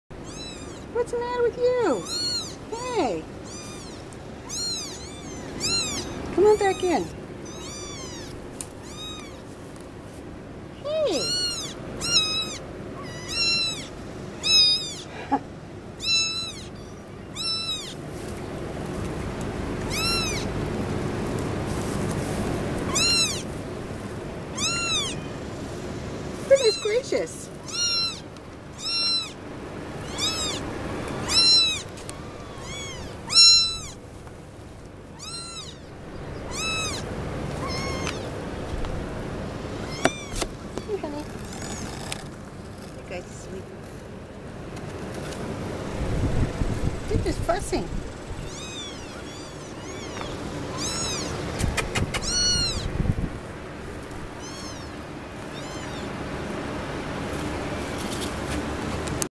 Minnie and her Munchkin kittens 5-16-11.MP4
Minnie's little black and white kittens - 4 are Munchkins.